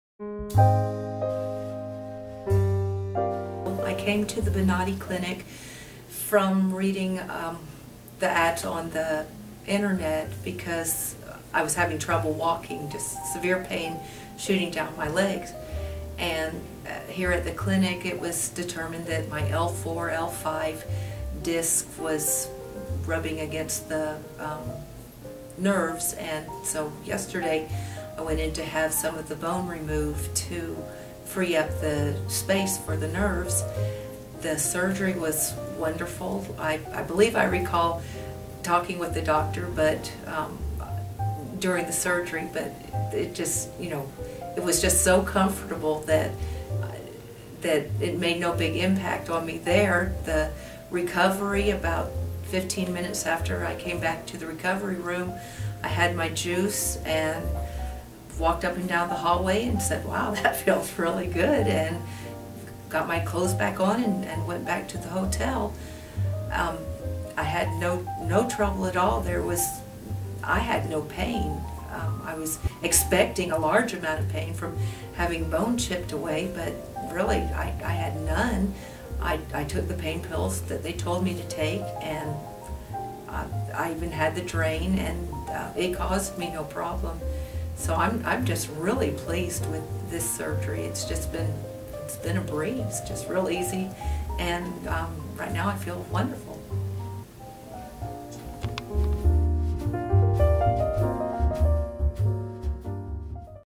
Ellenor S.: L4-L5; shooting pain into legs.
Area of Pain: Legs, L4- L5

Quote: "I'm just really pleased with this surgery. It's just been a breeze, just really easy and right now I feel wonderful.

Summary: "Ellenor came to the Bonati Spine Institute because of the severe pain shooting down her legs and having trouble walking. She was treated for issues in her L4-L5 disc level. Fifteen minutes after she came out of the surgery she was walking up and down the hallway and felt very good.
Despite her skepticism, she had no pain or trouble at all, and right now she feels wonderful.